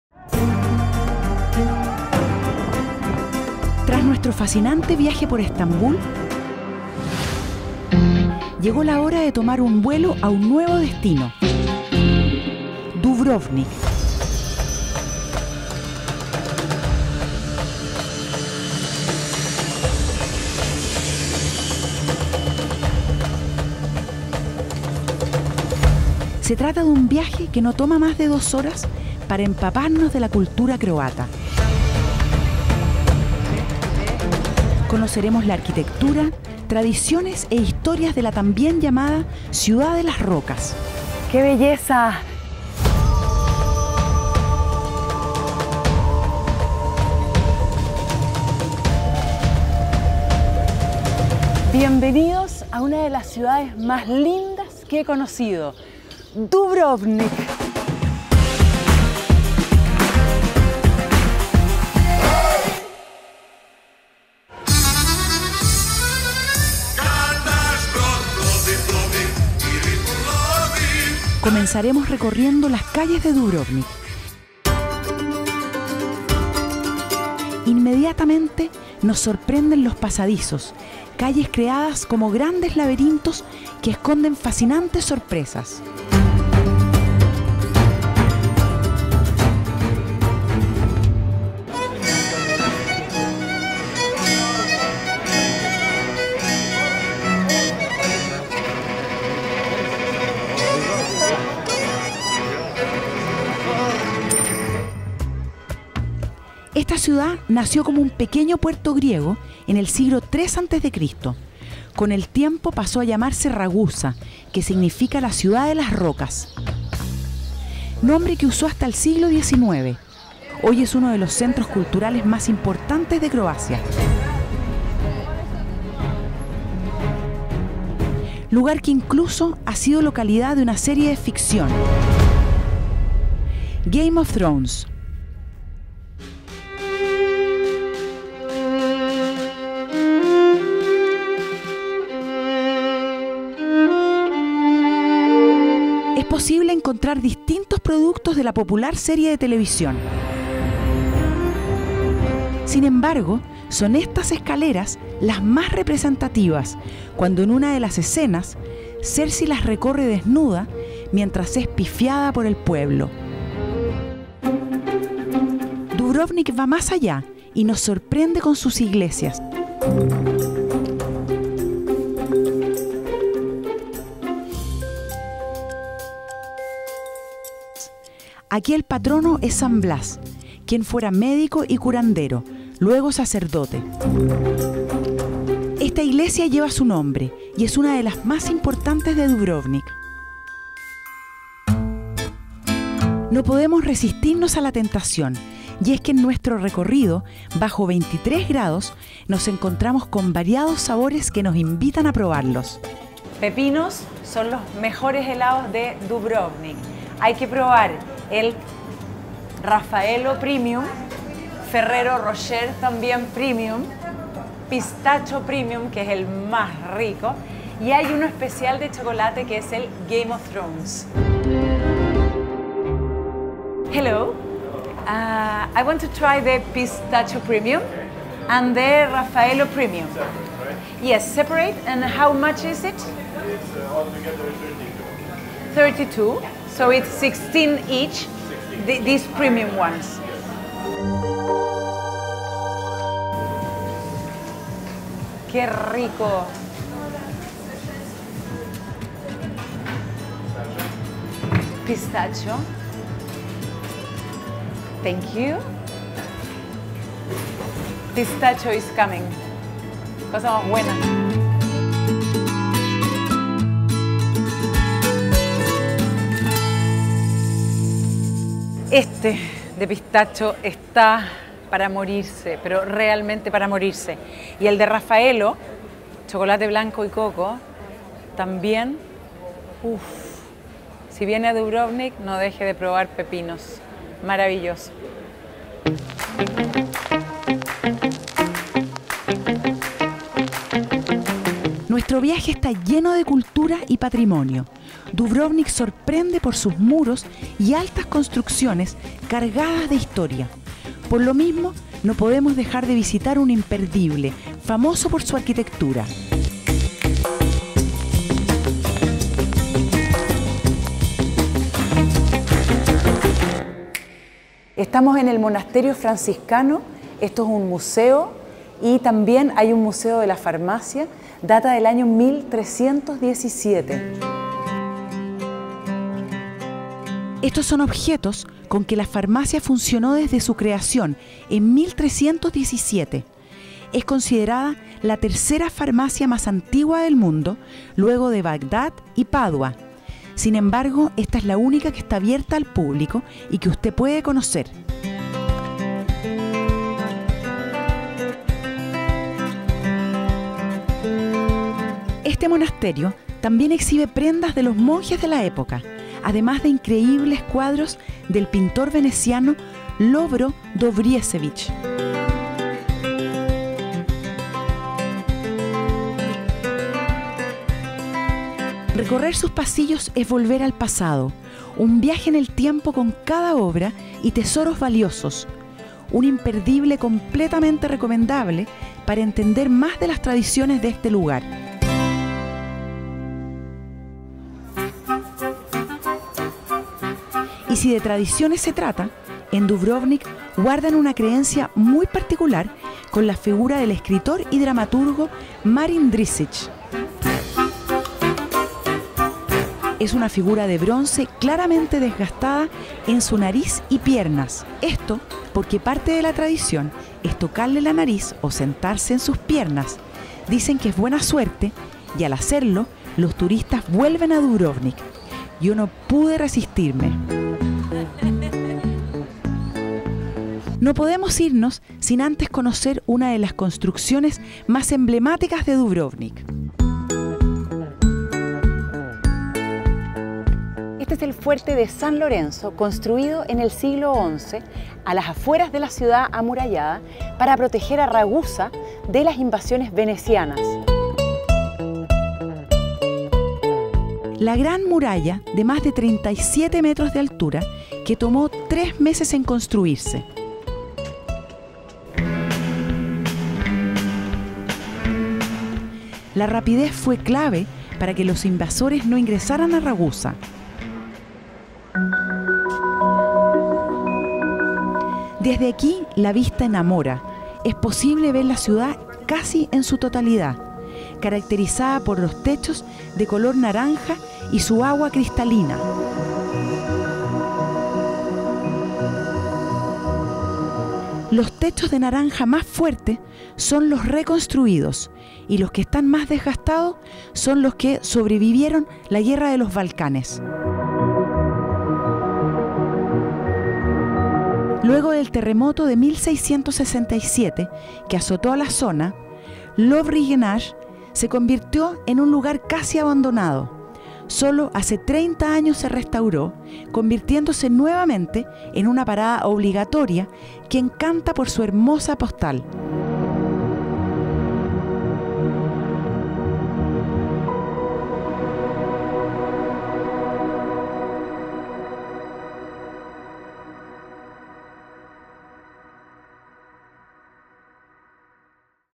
QUÉ VISITAR EN CROACIA I DUBROVNIK #01 ⚓ I La Gracia de viajar #16 ✈
Dubrovnik, la ciudad de los robles, la antigua Ragusa, república independiente por 450 años. Hoy, una de las ciudades amuralladas más bellas del mundo que invita a descubrir su fascinante y trágica historia. La reciente Guerra de los Balcanes es un capítulo digno de olvidar, pero que deja en evidencia la fortaleza y resistencia de este pueblo.